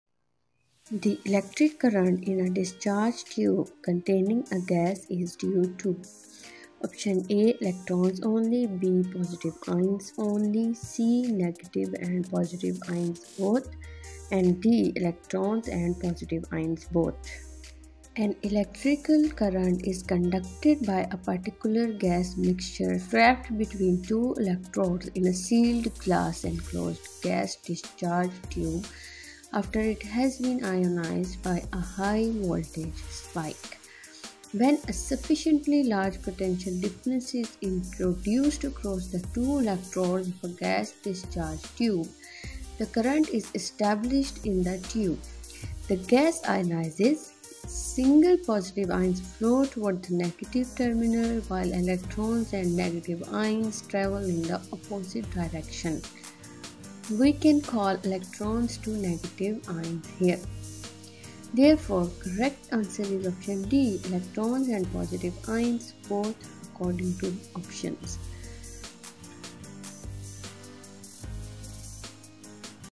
The electric current in a discharge tube containing a gas is due to ? Basic Physics Question 18
An electrical current is conducted by a particular gas mixture trapped between two electrodes in a sealed glass-enclosed gas discharge tube after it has been ionised by a high voltage spike.
When a sufficiently large potential difference is introduced across the two electrodes of a gas discharge tube, current is established in the tube. The gas ionises; single positive ions flow toward the negative terminal while electrons and negative ions travel in the opposite direction. We can call electrons to negative ions here. Therefore, correct answer is option D Electrons and positive ions both according to the options.